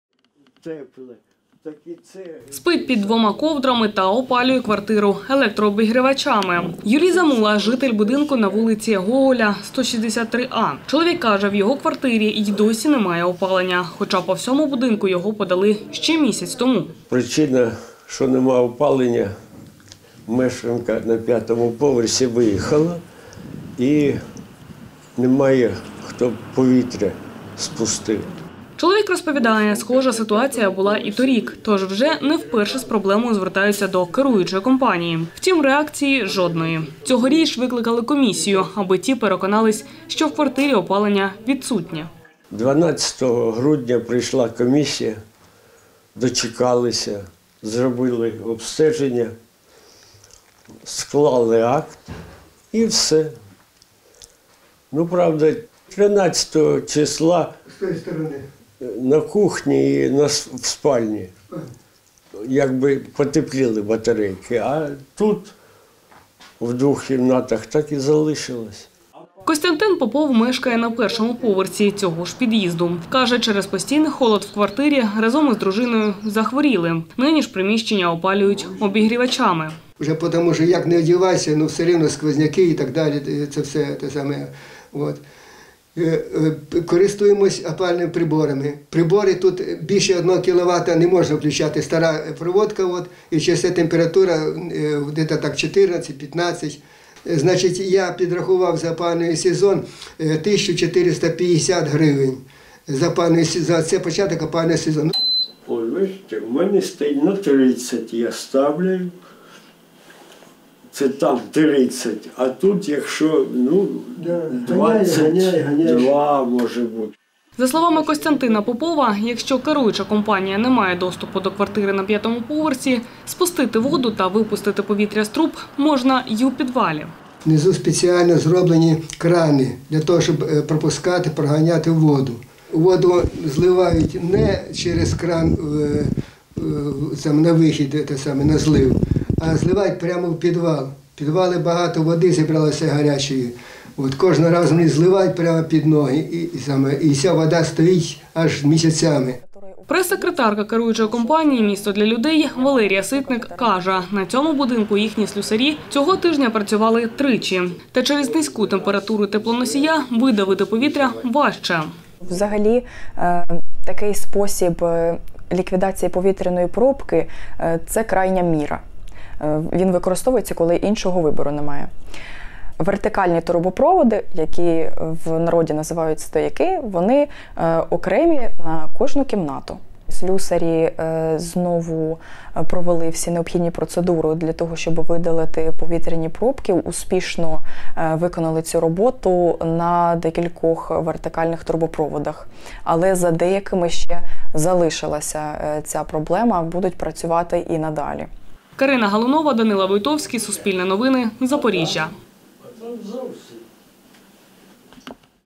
Жителі будинку на Гоголя 163-А скаржаться на відсутність тепла у квартирах
Вимушені опалюватись електроприладами, попри справні батареї. Житель будинку на вулиці Гоголя 163-А Юрій Замула каже, у нього в квартирі і досі холодно, хоча опалювальний сезон у місті стартував місяць тому. Така ж ситуація і у сусідів. У чому причина –  з’ясовували кореспонденти Суспільного.